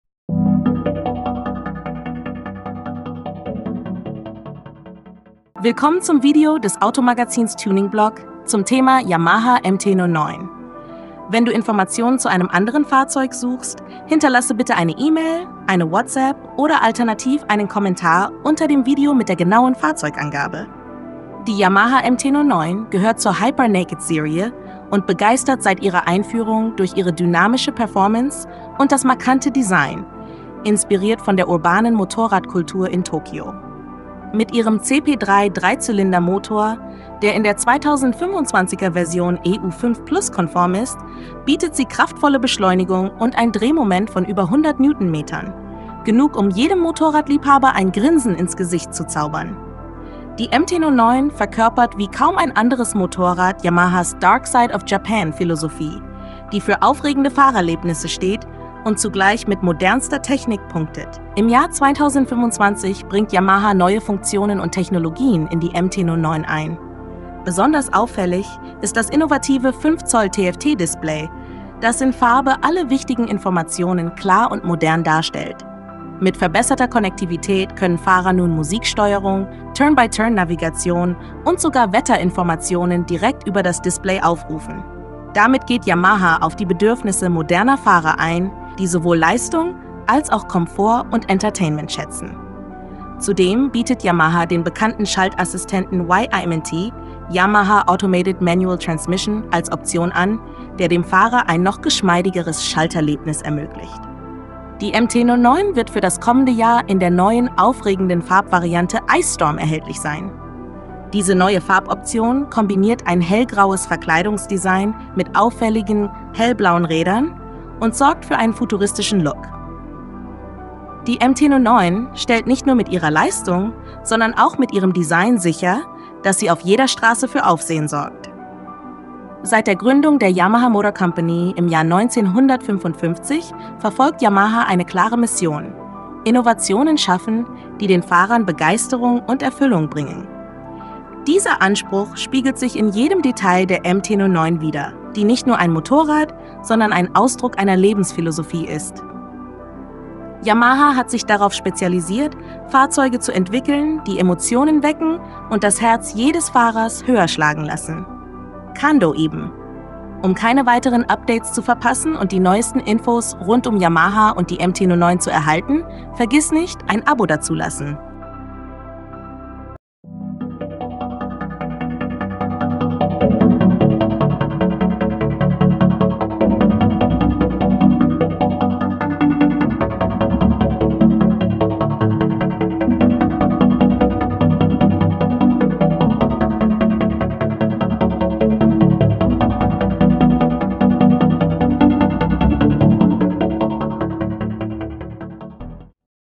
Yamaha MT-09 2025: Neues Design & Mega Performance | technische Details
✅ Source: Yamaha ➡️ Mehr in unserem Automagazin:

Entdecke die brandneue Yamaha MT-09 2025 mit ihrer kraftvollen Performance und dem frischen Look in "Ice Storm". Ausgestattet mit Yamahas CP3-Dreizylinder-Motor, beeindruckt die MT-09 durch starke Beschleunigung und ein Drehmoment von über 100 Newtonmetern. Die Hyper Naked Maschine kombiniert japanische Inspiration mit modernster Technik für ein aufregendes Fahrerlebnis.

Dank EU5+-konformem Motor, 5-Zoll-TFT-Display und wählbarer Yamaha Automated Manual Transmission (Y-AMT) bietet die MT-09 jetzt noch mehr Komfort und Kontrolle. Die neue Farboption "Ice Storm" und die markanten, hellblauen Räder setzen stylische Akzente und lassen das Bike überall herausstechen.

Tauche ein in die Dark Side of Japan und erlebe die MT-09 von Yamaha, die für 2025 mit noch mehr Power und Charakter aufwartet!

tuningblog - das Magazin für Auto-Tuning und Mobilität!